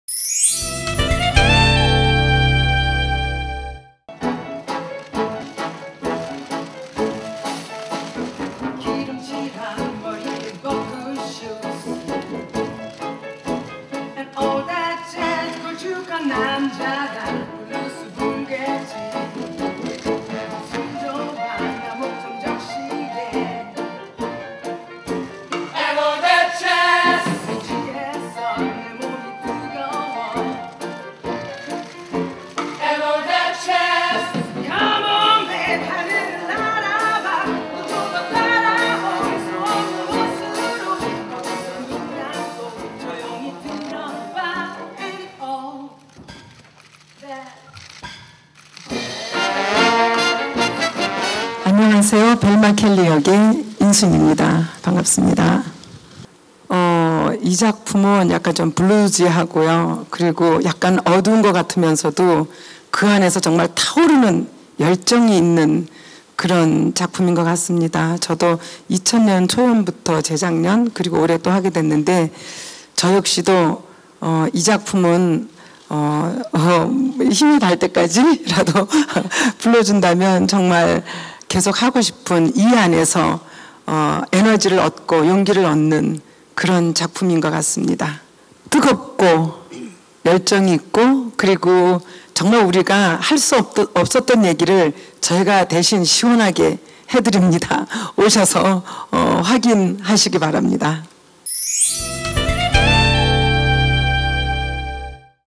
[SSTV] '시카고' 인순이 "힘 닿는 데까지 하고픈 작품"
[SSTV l 황예린 기자] 가수 인순이가 뮤지컬 '시카고'에 대한 애정을 드러냈다.

인순이는 8일 오후 서울 신도림 디큐브아트센터에서 열린 뮤지컬 '시카고' 프레스콜에 참석해 "'시카고'는 어두운 작품인 것 같지만 타오르는 열정이 있는 작품"이라고 말했다.

이어 인순이는 "'시카고'에는 2000년 초연 때 출연했던 작품이고 작년에 이어 올해 또 함께 하게 됐다. 힘 닿는데까지 오래오래 연기하고 싶다"고 덧붙였다.

2010년 공연 이후 2년 만에 관객들을 만나는 '시카고'는 오랜 시간 이 작품을 함께하며 작품의 깊이와 완성도를 높여왔던 배우들(인순이 최정원 남경주 성기윤 김경선 등)과 브로드웨이 오리지널 스태프들의 엄격한 심사를 통과한 아이비, 윤공주 그리고 에이모스 역의 류창우 등 새로운 배우들이 호흡을 맞췄다.

'시카고'는 1920년 보드빌 무대를 콘셉트로 심플하면서도 스타일리시한 무대를 선보인다. 또 무대 중앙에 14인조 빅밴드가 박칼린 음악감독의 지휘아래 '시카고'의 재즈 선율을 라이브 음악으로 선사한다.

한편 인순이 최정원 윤공주 아이비 남경주 성기윤 김경선 류창우가 출연하는 뮤지컬 '시카고'는 오는 10일부터 10월 7일까지 디큐브아트센터에서 공연된다.